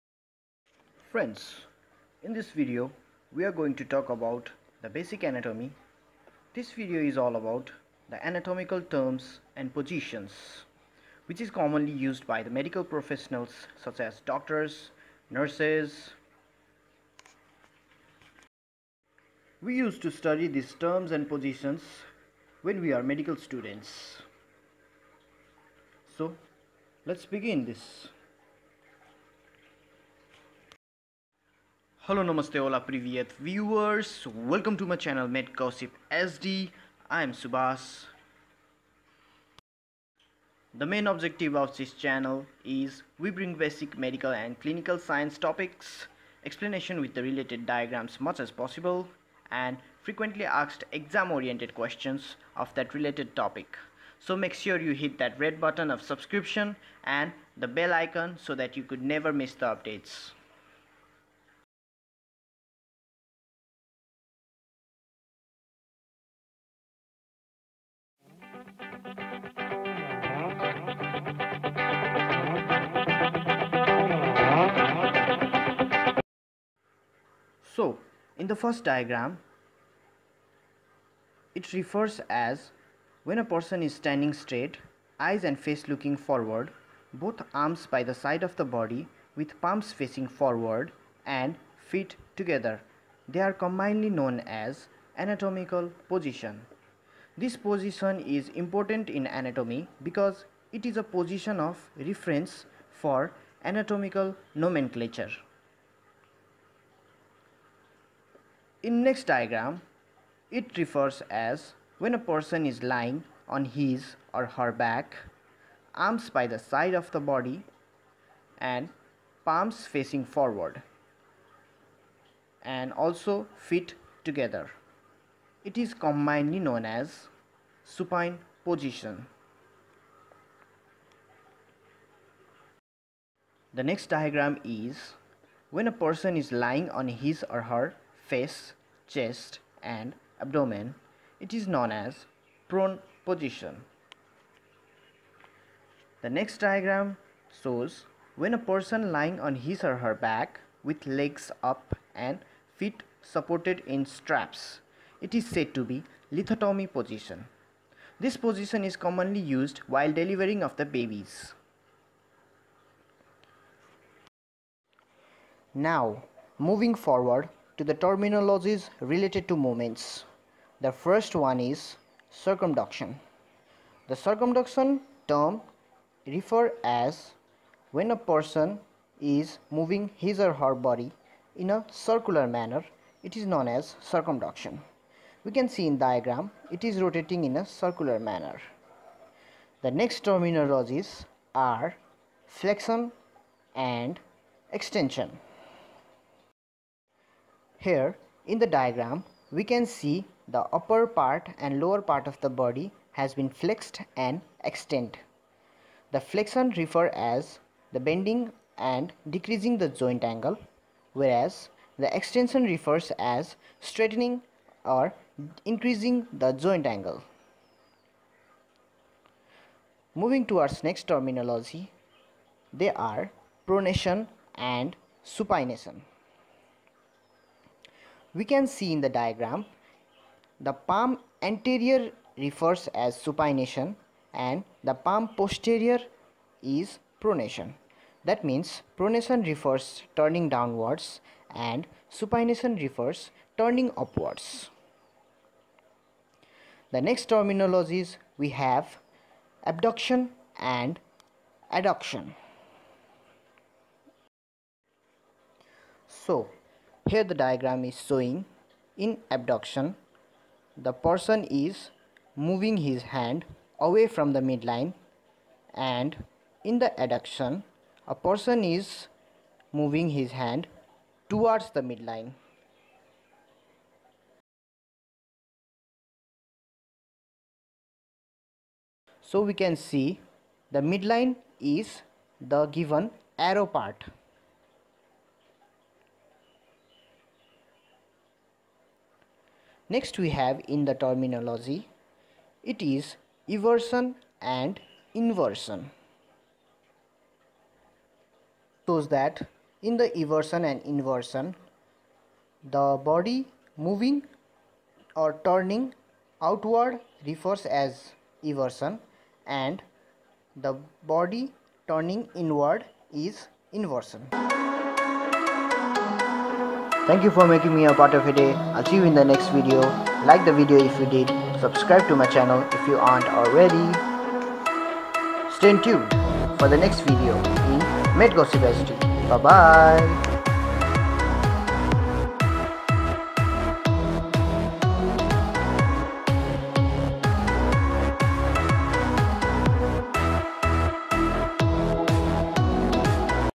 Anatomy terminologies and position in english- basic definition with diagram
Hello viewers, this is very basic video which  is all bout the common terminologies and positions used in anatomy for understanding the location of body easily :)
please like the video if u did and please support my channel by subscribing if you enjoy the video.. i will bring more and more updates if u guys motivated me